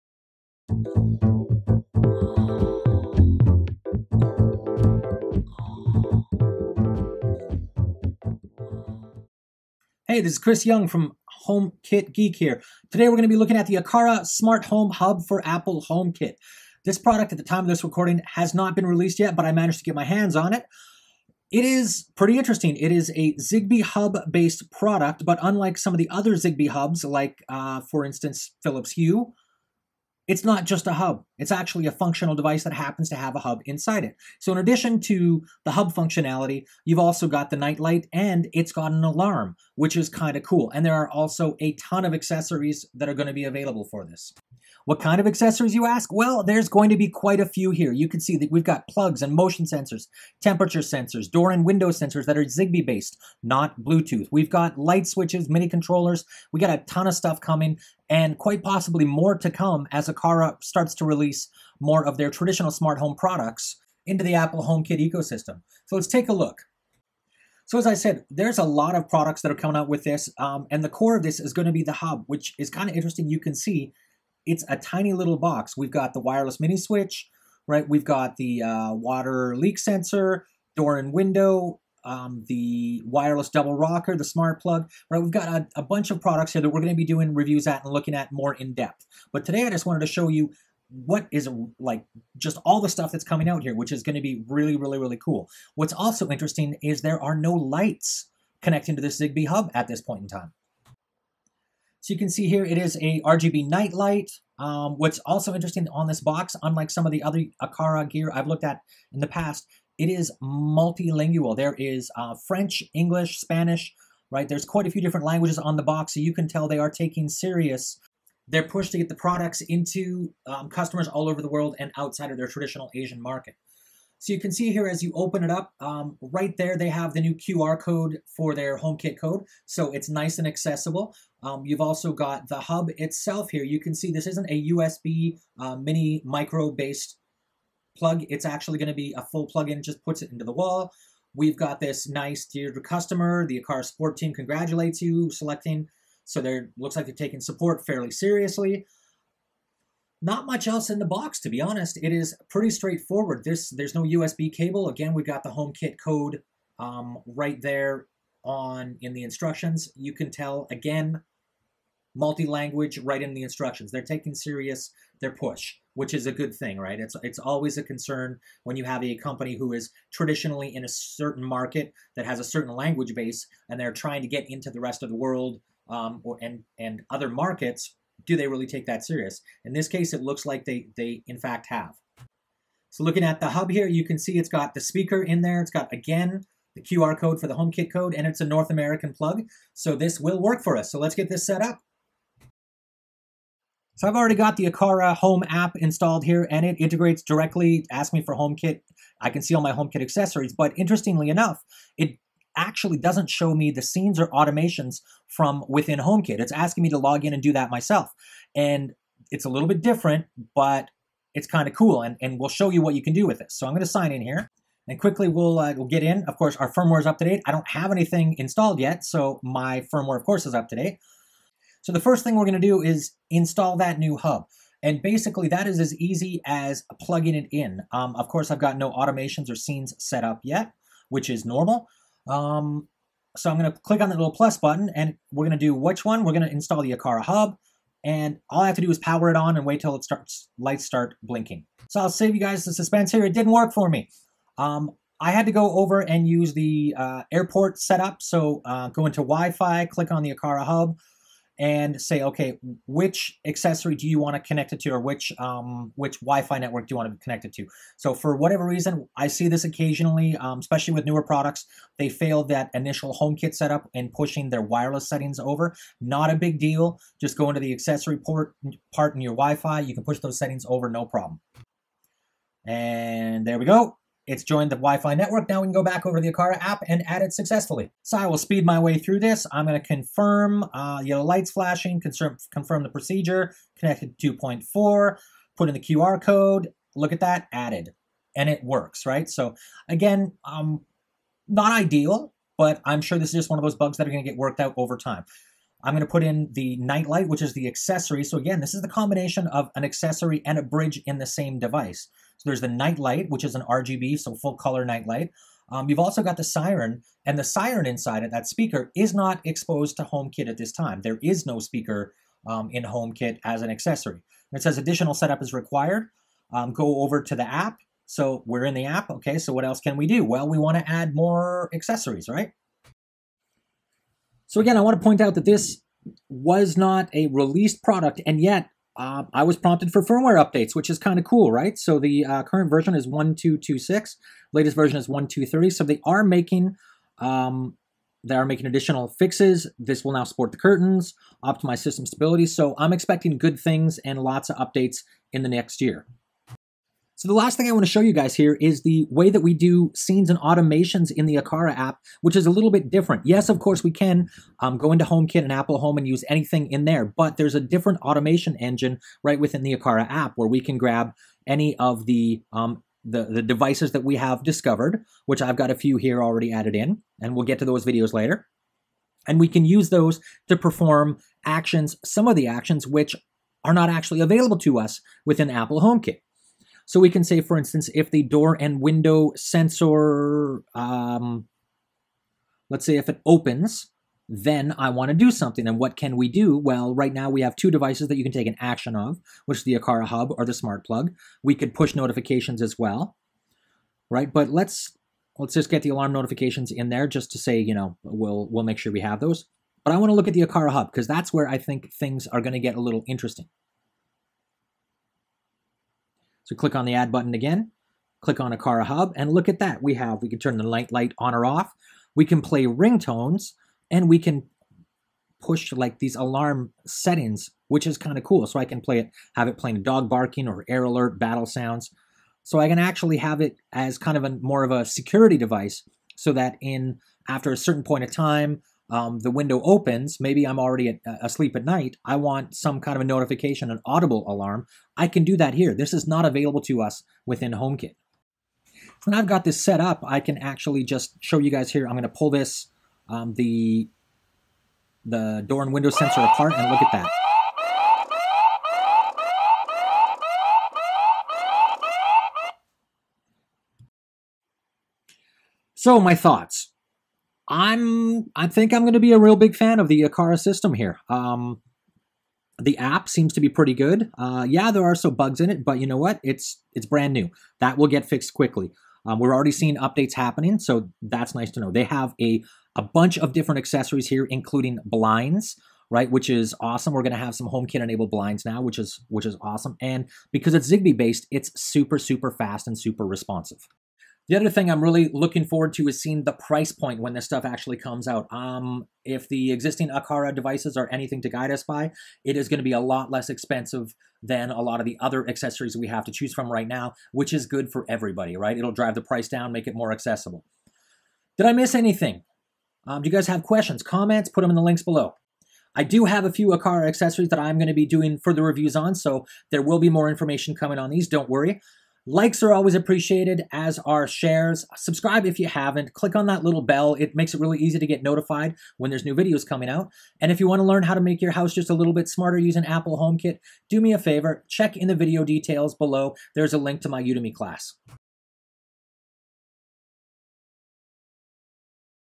Aqara Homekit Hub: First Looks
Asian Smarthome Vendor Aqara is releasing a new line of SmartHome products focused with Apple Homekit support. I was lucky enough to have Aqara send me some gear to look at and I have to say I'm really impressed.  Aqara took their requiremnet of having a zigbee based bridge and made it an advantage of their system by building it into a combination nightlight and alarm with multiple sound choices. There are various zigbee based accessories which the Aqara hub supports and we'll be posting additional videos over the next few weeks as we get a chance to work with the Aqara Smart Home system.